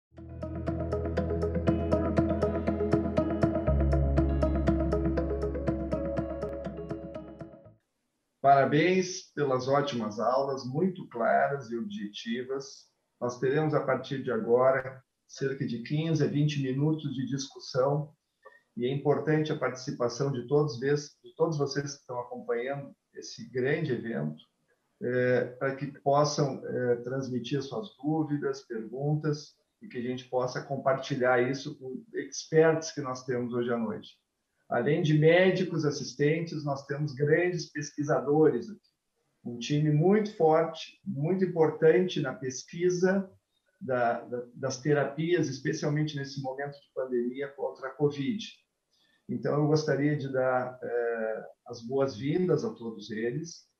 COVID-19: Anticoagulação Profilática ou Terapêutica ?
Neste debate os  médicos, Paulo Behr, Eduardo Ramacciotti, Renato Lopes, Pedro Barros, discutem os esquemas mais adequados de acordo com as evidências disponíveis